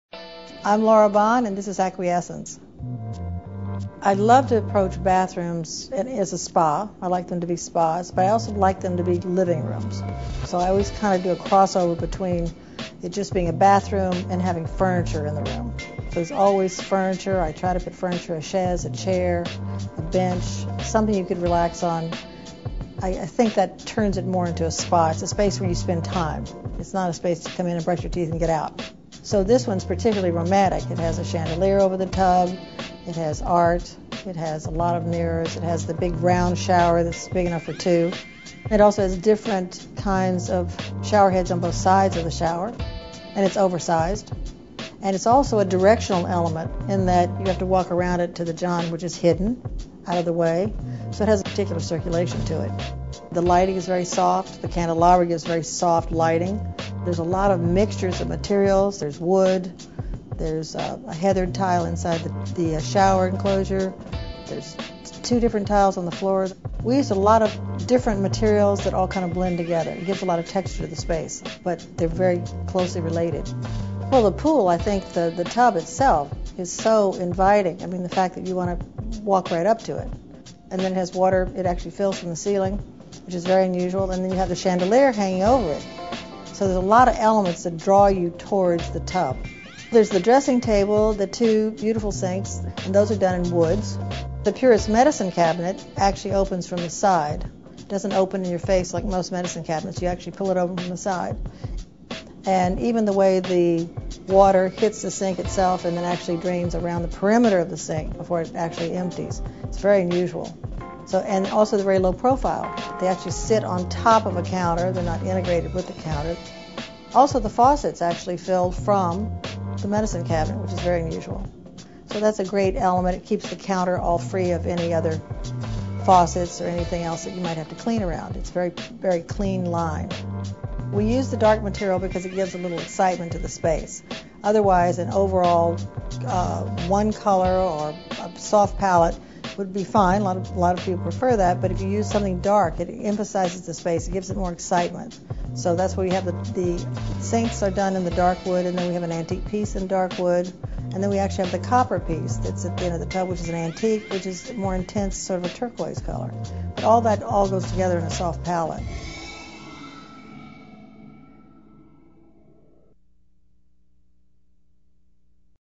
Aquaessence
Designer Laura Bohn discusses her vision for this tranquil, spa-style bath.